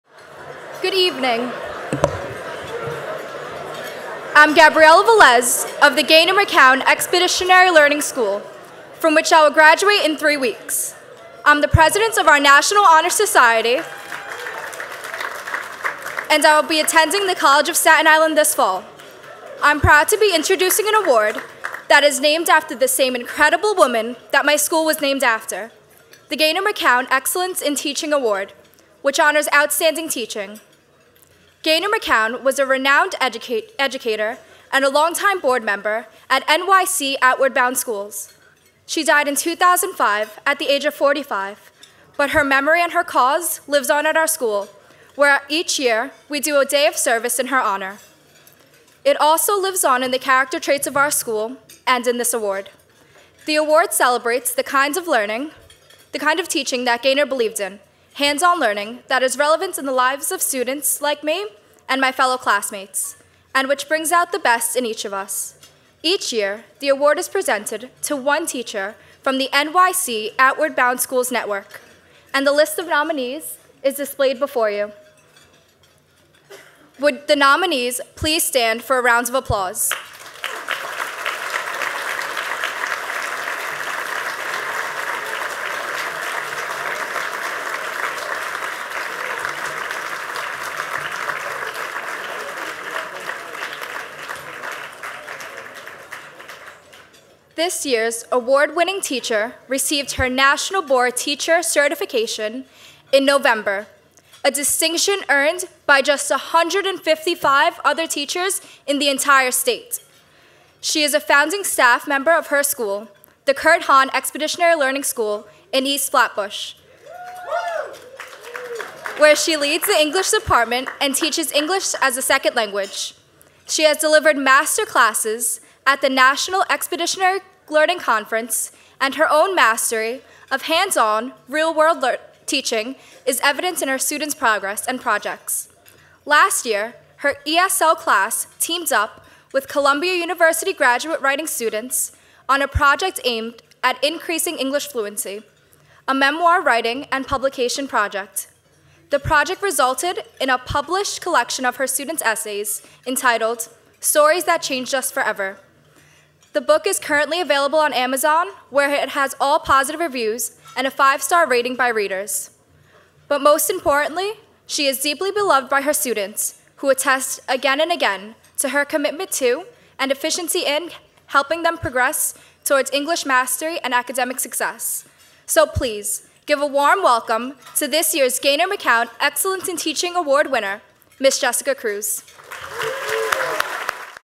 Senior Gabriela Velez Introduces Gaynor McCown Award-Winning Teacher
Senior Gabriela Velez of our Gaynor McCown Expeditionary Learning School introduces our 2013 Gaynor McCown Excellence in Teaching Award Winner: Jessica Kruse, ESL teacher, of our Kurt Hahn Expeditionary Learning School in East Flatbush, Brooklyn.